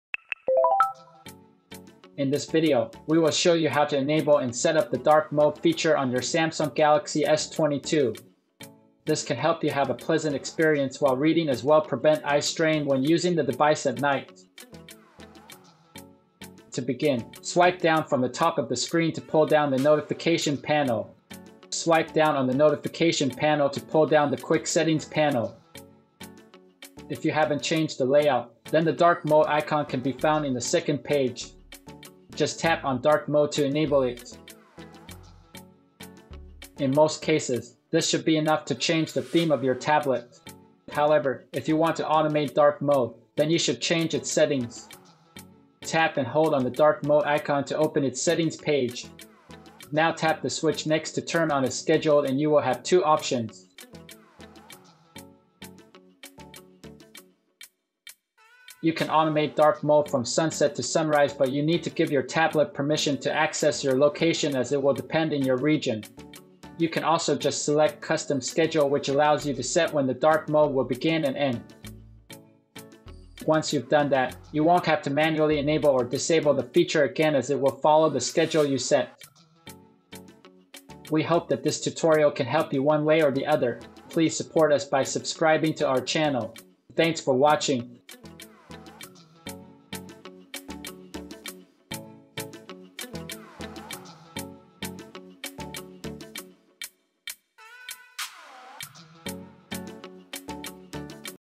How To Enable & Set Up Dark Mode On Samsung Galaxy S22/S23
Unlock the sleek and stylish Dark Mode on your Samsung Galaxy S22/S23 with our step-by-step guide! In this video tutorial, we'll show you how to enable and set up Dark Mode on your device, allowing you to enjoy a visually stunning and battery-saving display.

Discover the benefits of Dark Mode, from reducing eye strain to extending battery life. Follow our expert instructions as we navigate through the settings menu, demonstrating how to customize Dark Mode to suit your preferences.

Say goodbye to bright backgrounds and embrace the elegant darkness on your Samsung Galaxy S22/S23. Watch now and elevate your user experience with Dark Mode!